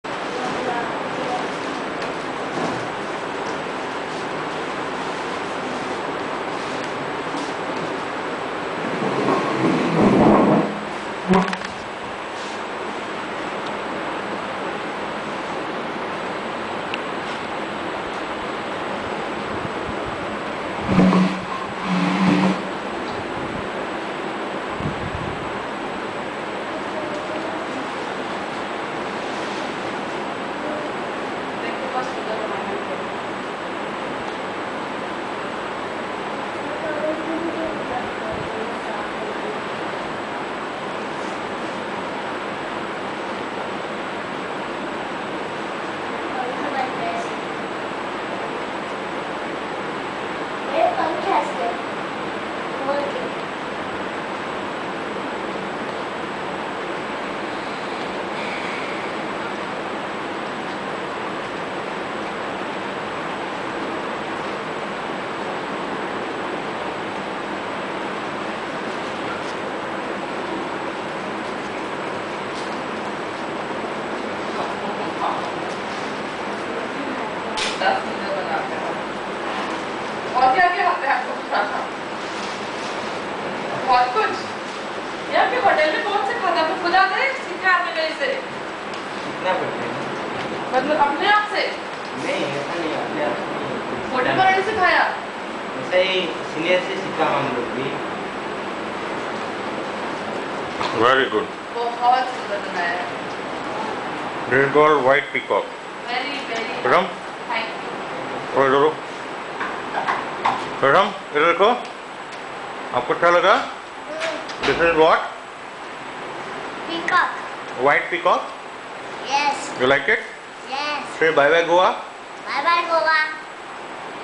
MAKING OF WHITE PEACOCK WITH TOWEL.
MAKING OF WHITE PEACOCK WITH TOWEL...GOOD CREATION BY HOTEL GUY IN GOA.
recorded by SANDEEP HANDA